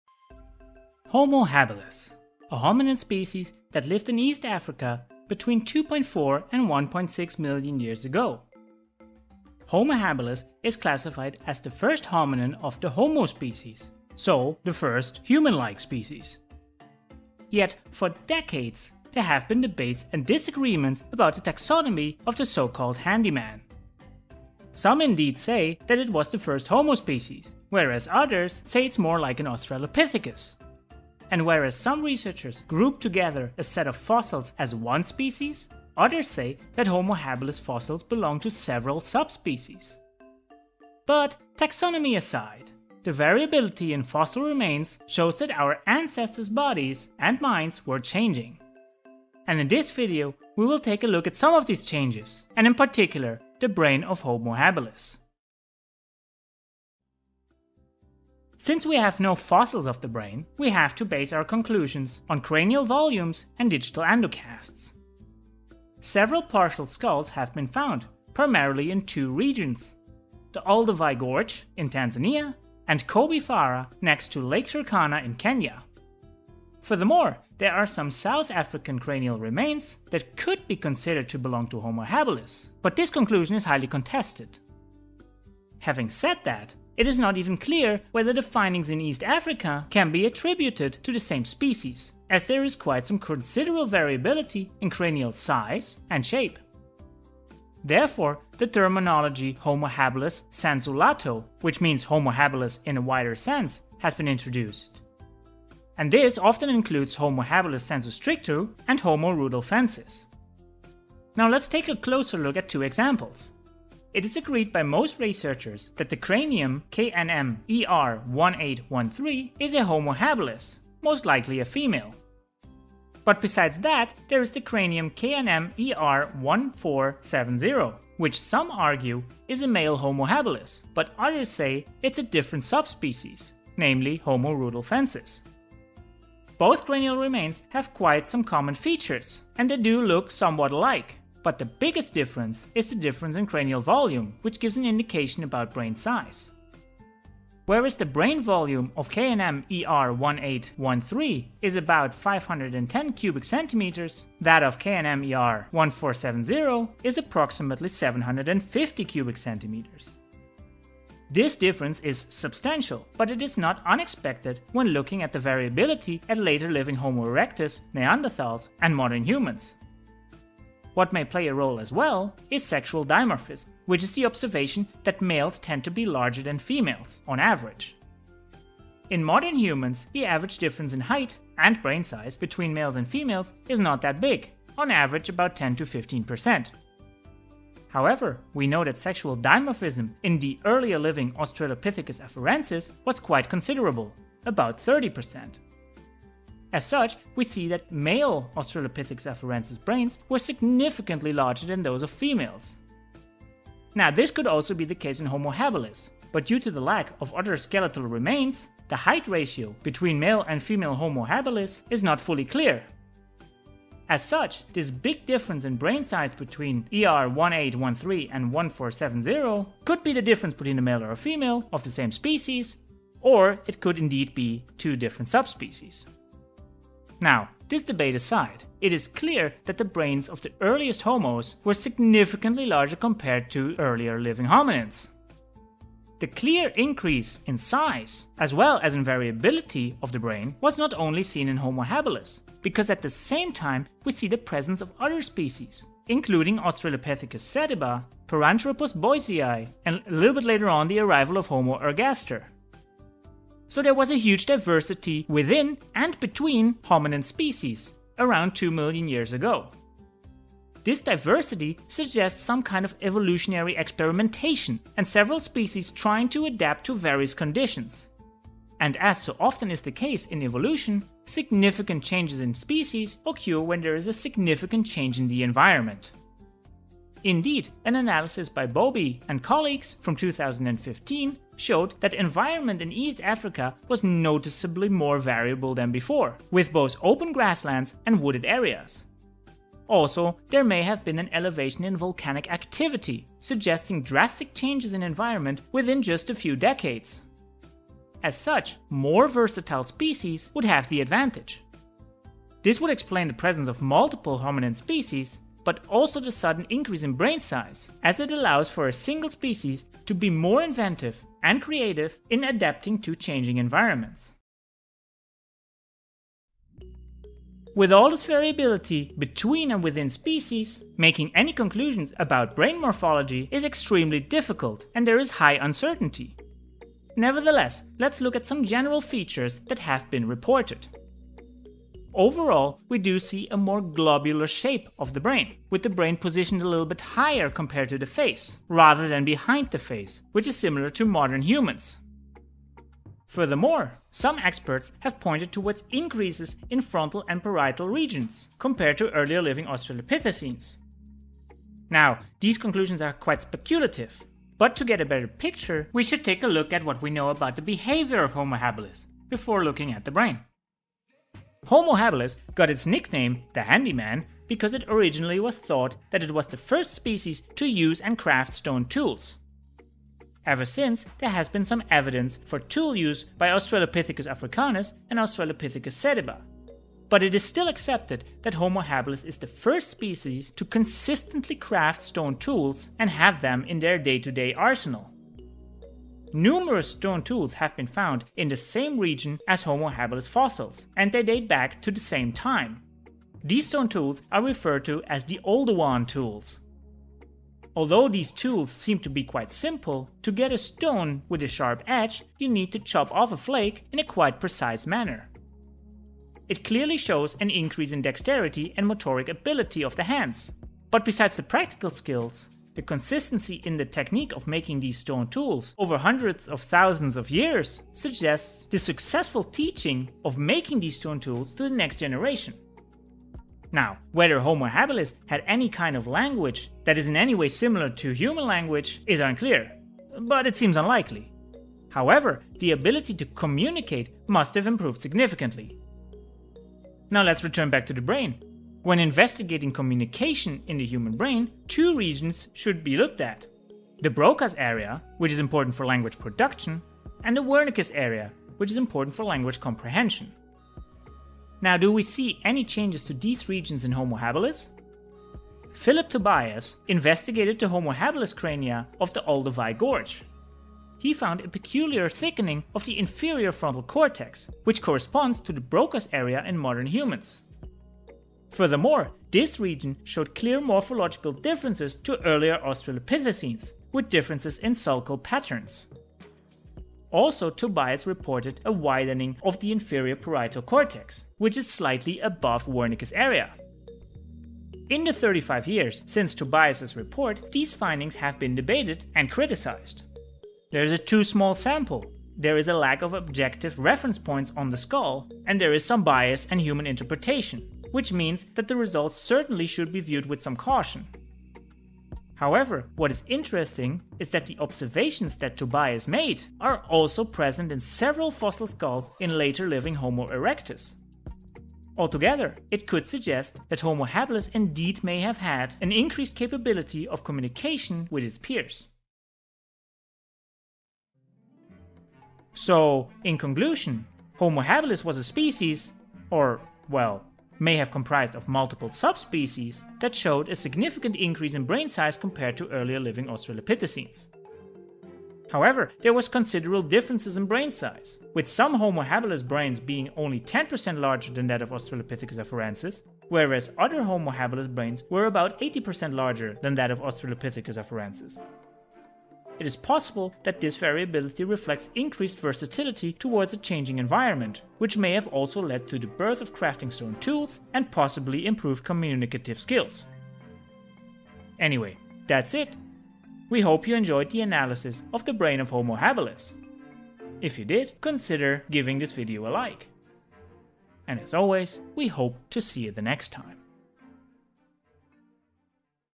The Brain of Early Homo (Homo Habilis & Homo Rudolfensis) | Ancestors Brains #4
Homo habilis is an early human species living between 2.4 and 1.6 million years ago. In this video we take a look at their brain.

Homo habilis, the first homo species forms a transitional species between australopithecus and homo erectus. When looking at the brain we see a more rounded shape compared to earlier hominids. Also the brain is positioned higher, above eye-level, whereas in australopithecines the brain is found behind the face. Also the brain is significantly larger compared to australopithecus.

However, there is a huge amount of variability. Early homo have brain sizes ranging from 500 cc to almost 800 cc. Therefore, some suggest that homo habilis (sensu lato) comprises of multiple subspecies, like homo habilis sensu stricto, homo rudolfensis, and homo gautengensis. Others have suggested that the brain size variability is normal and expected within a single species. Possible size differences may also be explained by sexual dimorphism.

Homo habilis is known to use and craft stone tools. It is highly likely that the communicative skills of handy man also improved, to share these skills with the next generation. Interestingly, Tobias (1987) observed more pronounced language areas (Broca's area and inferior parietal cortex) in homo habilis. Although not uncontested, these findings are in line with what we see in later homo erectus.

Narrated by: Miles Wischnewski

References:
Balzeau, A., Holloway, R. L., & Grimaud-Hervé, D. (2012). Variations and asymmetries in regional brain surface in the genus Homo. Journal of human evolution, 62(6), 696–706.

Benazzi, S., Gruppioni, G., Strait, D. S., & Hublin, J. J. (2014). Technical note: virtual reconstruction of KNM-ER 1813 Homo habilis cranium. American journal of physical anthropology, 153(1), 154–160.

Bobe, R., & Carvalho, S. (2019). Hominin diversity and high environmental variability in the Okote Member, Koobi Fora Formation, Kenya. Journal of human evolution, 126, 91–105.

Broadfield, D. C., Holloway, R. L., Mowbray, K., Silvers, A., Yuan, M. S., & Márquez, S. (2001). Endocast of Sambungmacan 3 (Sm 3): a new Homo erectus from Indonesia. The Anatomical record, 262(4), 369–379.

Bruner, E., & Beaudet, A. (2023). The brain of Homo habilis: Three decades of paleoneurology. Journal of human evolution, 174, 103281.

Curnoe, D., & Tobias, P. V. (2006). Description, new reconstruction, comparative anatomy, and classification of the Sterkfontein Stw 53 cranium, with discussions about the taxonomy of other southern African early Homo remains. Journal of human evolution, 50(1), 36–77.

Lieberman, D.E., Pilbeam, D.R., Wood, B.A., (1988). A probabilistic approach to the problem of sexual dimorphism in Homo habilis: a comparison of KNM-ER 1470 and KNM-ER 1813. Journal of Human Evolution, 17(5), 503-511.

Miller J. A. (1991). Does brain size variability provide evidence of multiple species in Homo habilis?. American journal of physical anthropology, 84(4), 385–398.

Spoor, F., Gunz, P., Neubauer, S., Stelzer, S., Scott, N., Kwekason, A., & Dean, M. C. (2015). Reconstructed Homo habilis type OH 7 suggests deep-rooted species diversity in early Homo. Nature, 519(7541), 83–86.
Tobias, P.V. (1987). The brain of Homo habilis: A new level of organization in cerebral evolution. Journal of Human Evolution, 16(7-8), 503-511.

SECOND CHANNEL - Psyched! Discussions